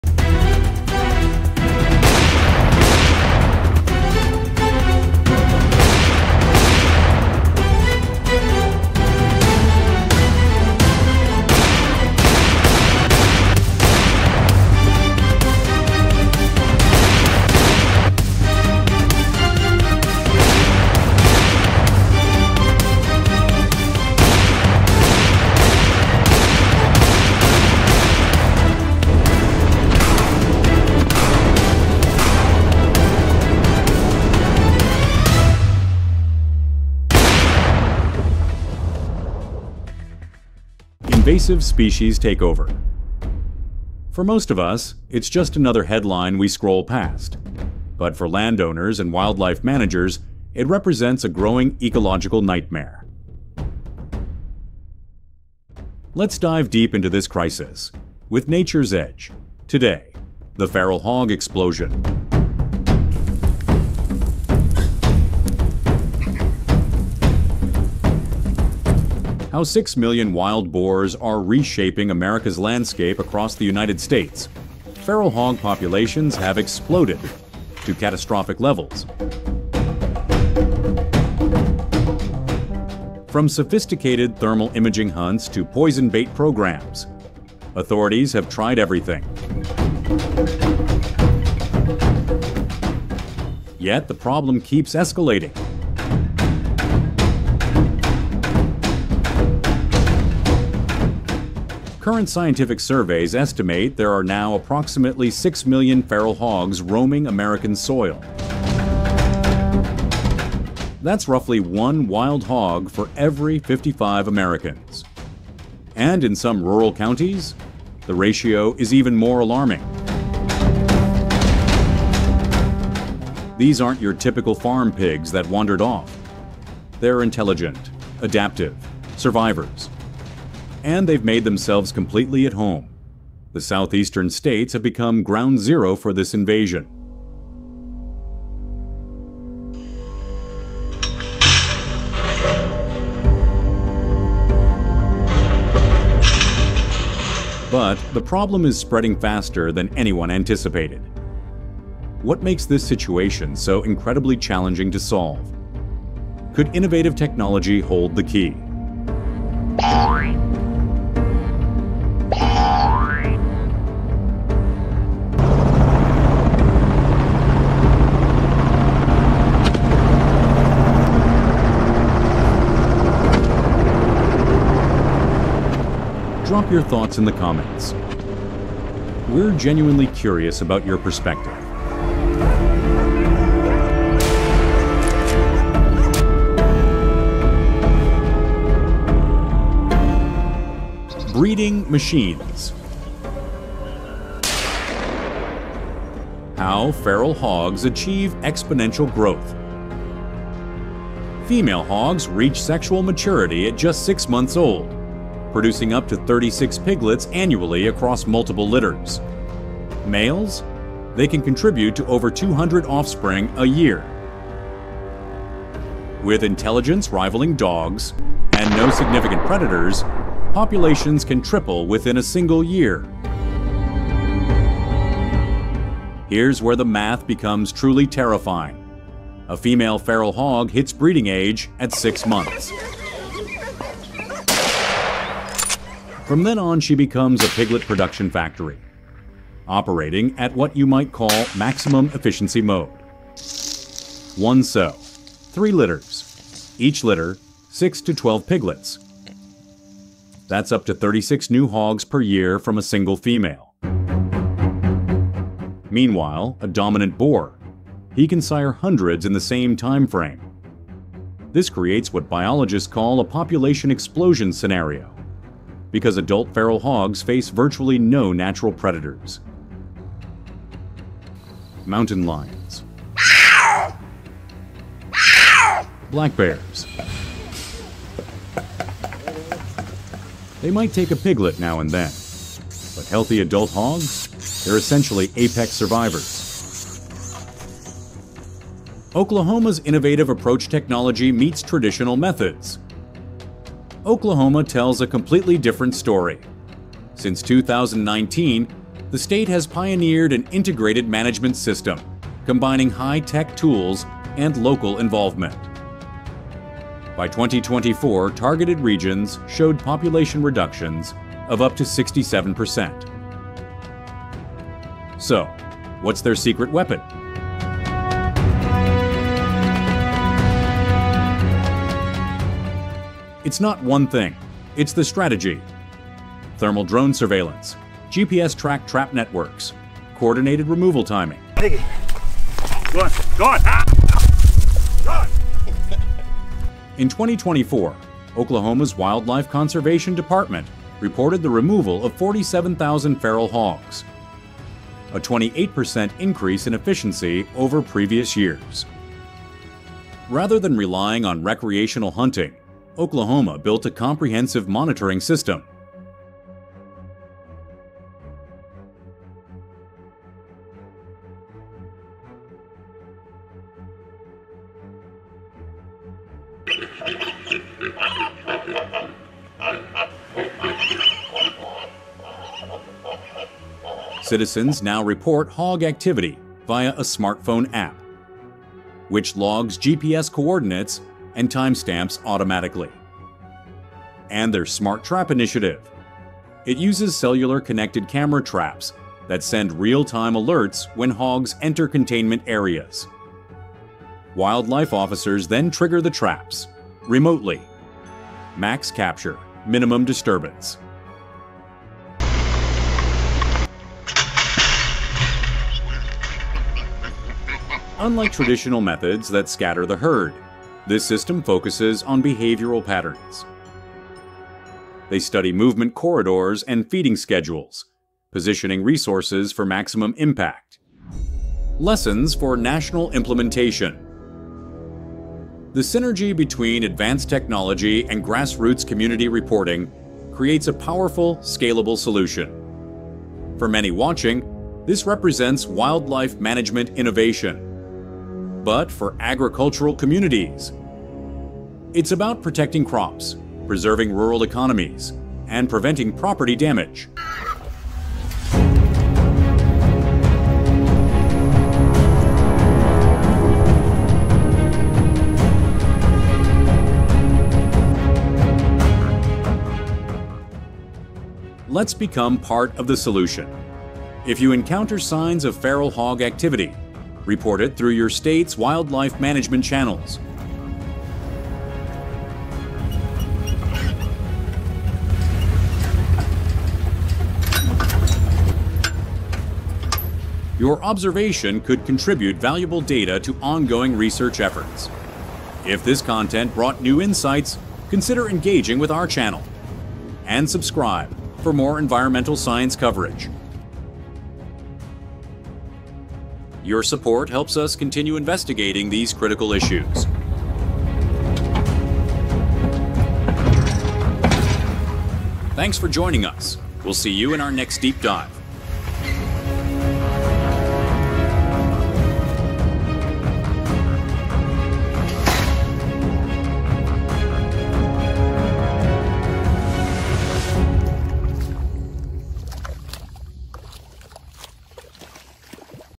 Oklahoma’s Stunning 84% Reduction in Wild Boars WITHOUT Shooting!
Feral hogs are wreaking havoc across America — destroying farmland, disrupting ecosystems, and spreading fast. With over 6 million wild pigs on the loose, states are desperate for solutions.

But one state is making serious progress.

Oklahoma has taken a groundbreaking approach: combining thermal drone surveillance, GPS-linked smart traps, and community reporting tools. The result? A stunning 67% reduction in targeted areas — and over 47,000 hogs removed in just one year.

Unlike recreational hunting, this strategy focuses on science, timing, and behavioral patterns — and it’s working.

Is this the future of invasive species control?